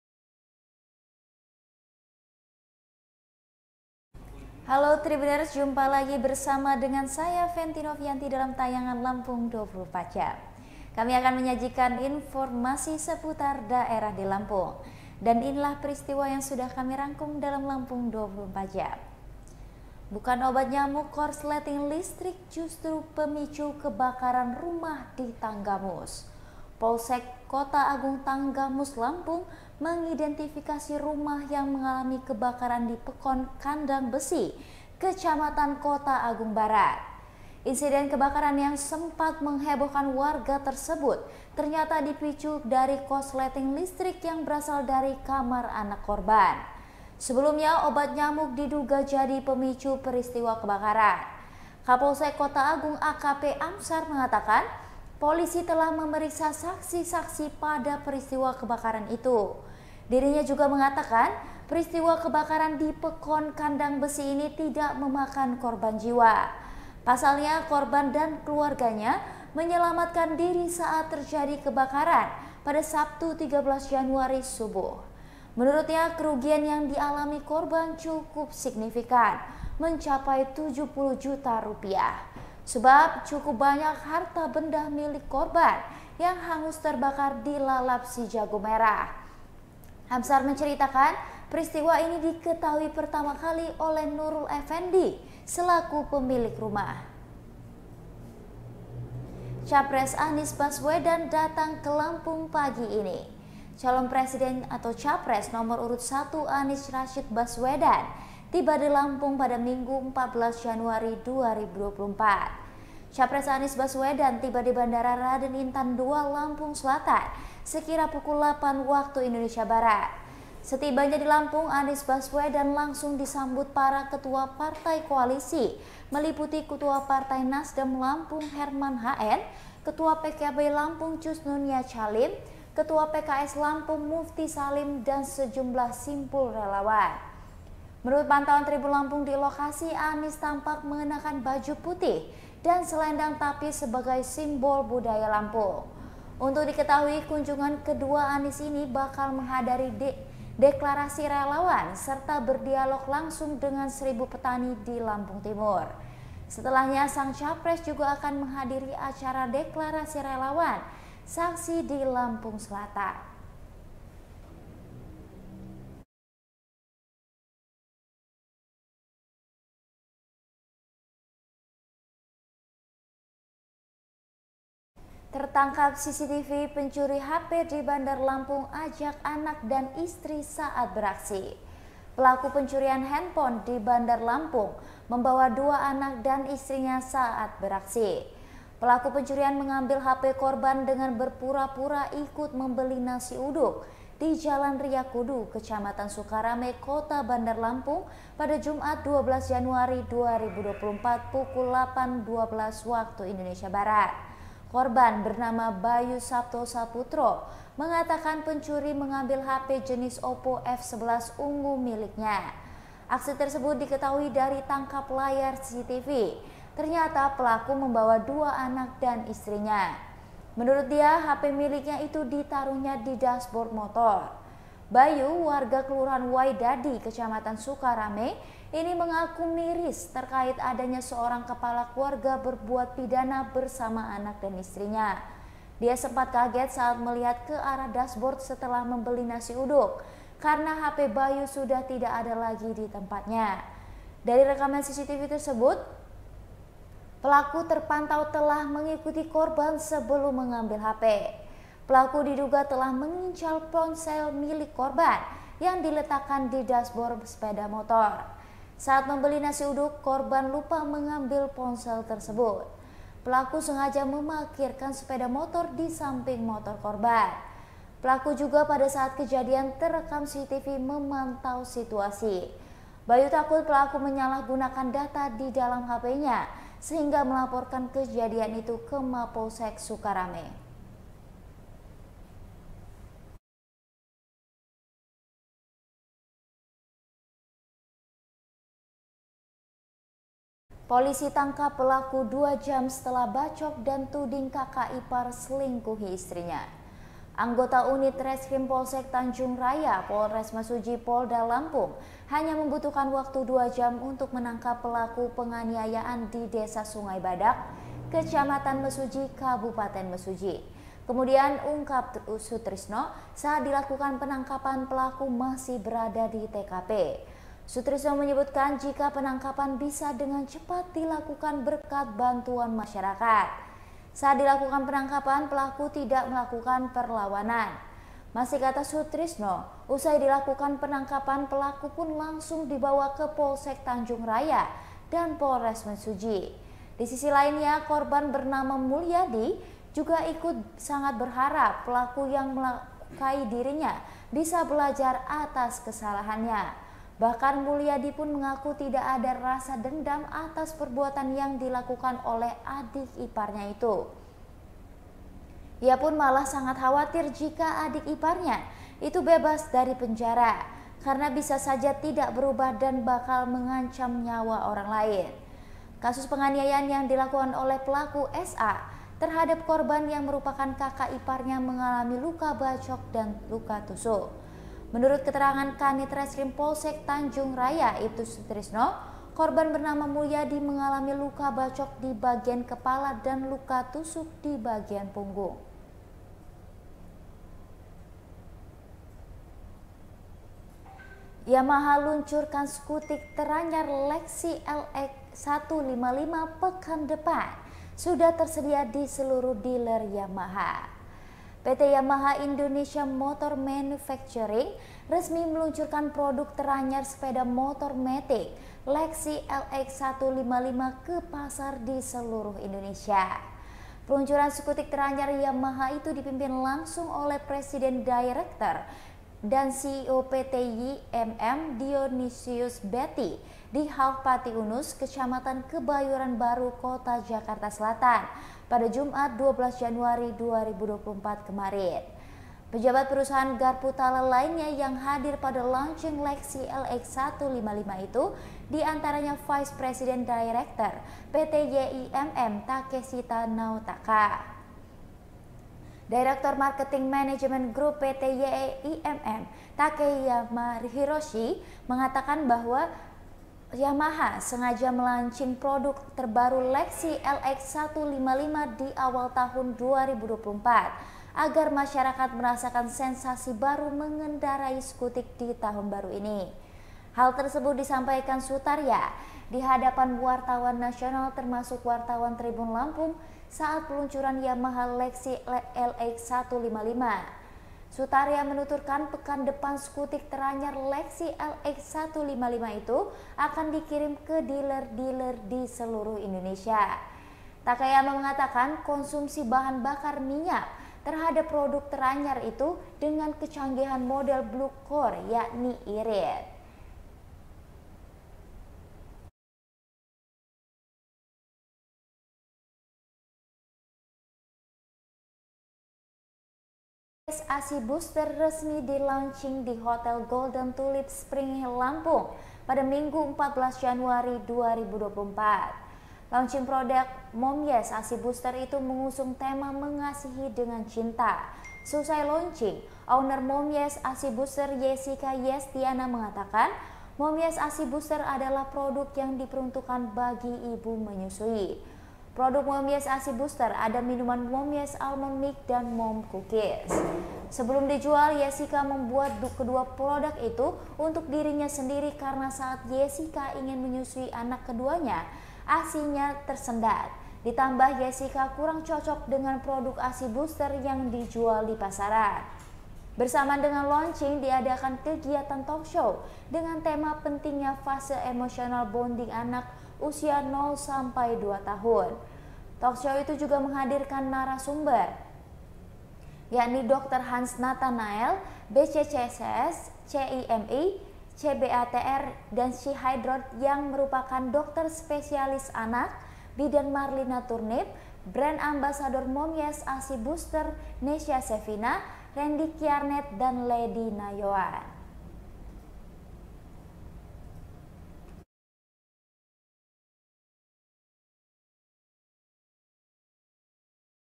Bukan Obat Nyamuk, Korsleting Listrik Justru Pemicu Kebakaran Rumah di Tanggamus
Tim SAR Bandar Lampung Tutup Pencarian Balita Terseret Air Hujan